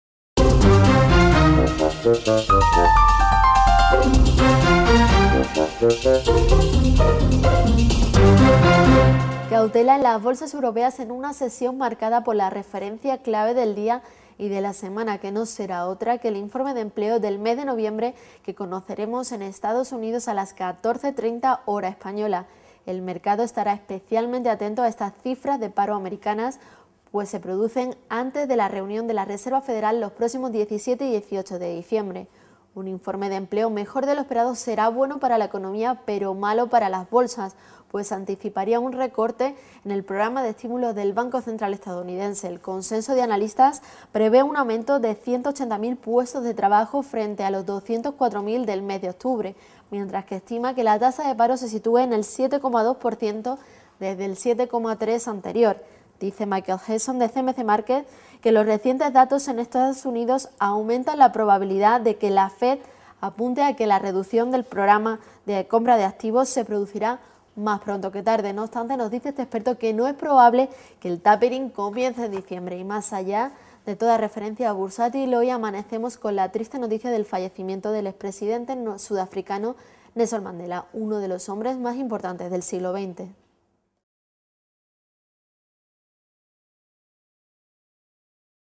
Claves del día
Todo gira en torno a una hora y un dato. A las 14:30, hora española, se conocerá el Informe de Empleo de noviembre en Estados Unidos, una referencia que los inversores "temen" que sea demasiado buena. Se impone de nuevo la máxima de que "buenas noticias son malas noticias", al menos para las bolsas. Unos datos mejores de lo esperado podrían ser indicador de que el tapering de la Reserva Federal (Fed) podría llegar más pronto que tarde.